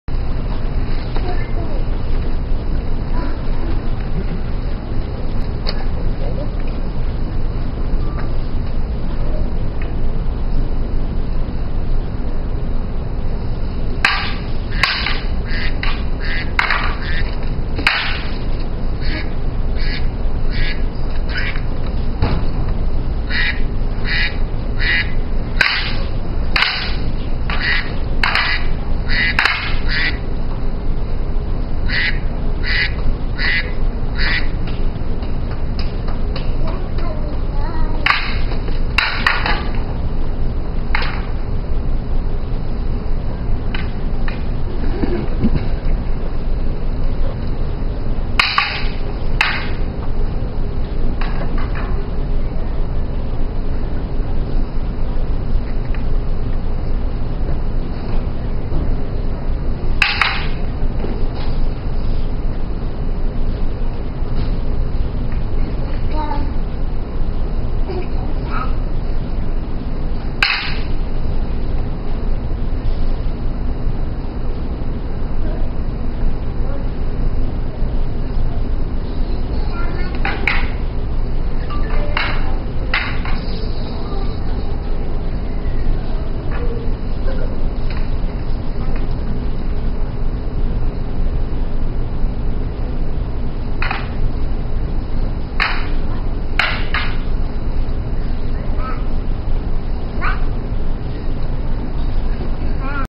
Aikido Kumitachi demonstration by young adult students at Aki Matsuri festival in 2011
Aikido Northshore dojo demonstration at Bellevue College for Aki Matsuri 2011. Kumitachi is the samurai Japanese sword techniques practiced with wooden swords called bokken.